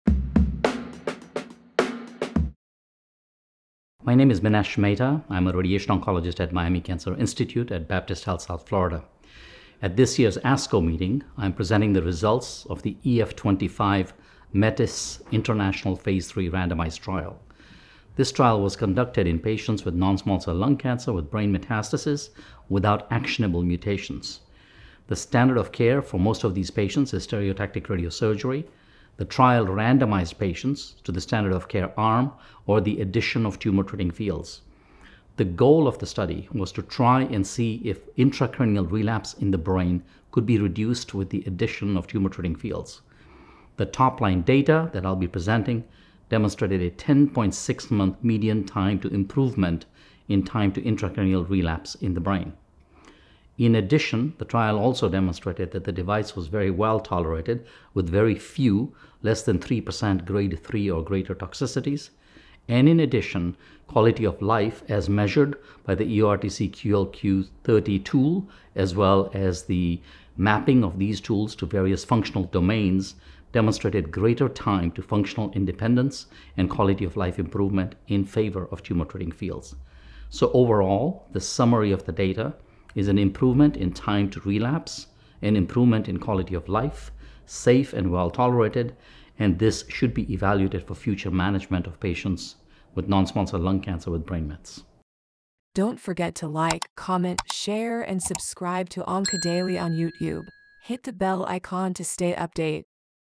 Dr. Minesh P Mehta on Breakthrough in Brain Metastasis Treatment for Lung Cancer | ASCO 2024
Dr. Minesh P Mehta, a radiation oncologist at Miami Cancer Institute, discusses his presentation at the ASCO meeting on the EF25 METIS Phase III trial. This study focused on patients with non-small cell lung cancer with brain metastases, exploring the addition of tumor treating fields to standard stereotactic radiosurgery. The trial showed a 10.6-month improvement in time to intracranial relapse, along with well-tolerated treatment and better quality of life for patients.